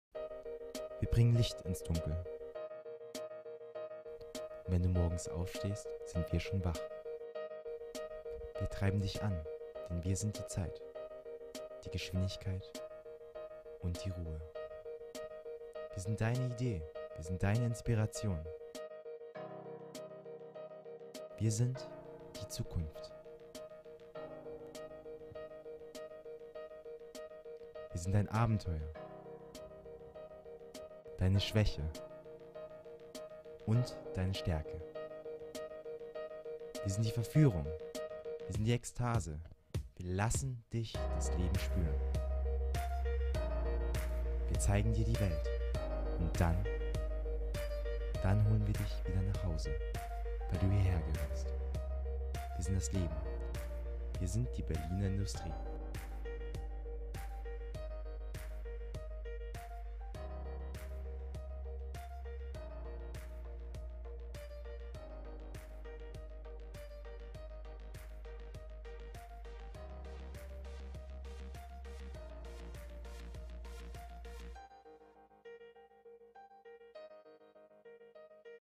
Wir sind die Berliner Industrie!
SRH Hochschule für Kommunikation und Design
Bachelor Film + Motion Design
B.A. Kommunikationsdesign &
B.A. Marketingkommunikation
Audiovisuelle Kommunikation
Projekt: Berliner Industrie in Kooperation mit IHK Berlin, beBerlin und Berliner Senatsveraltung
WS 2014/15